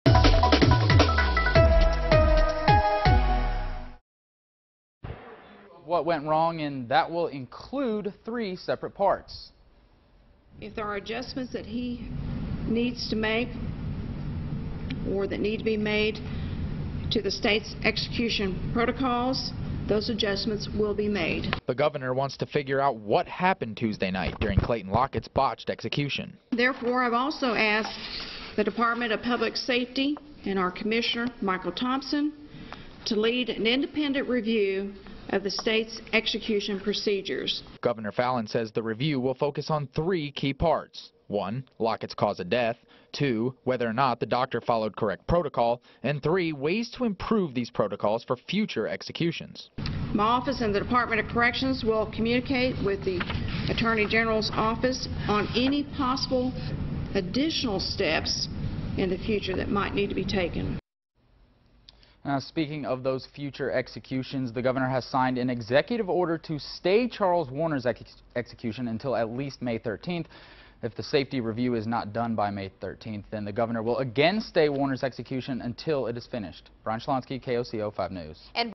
Gov. Fallin orders independent review into botched execution
Oklahoma Gov. Mary Fallin is calling for an independent review of the state's execution protocols after Tuesday's botched execution.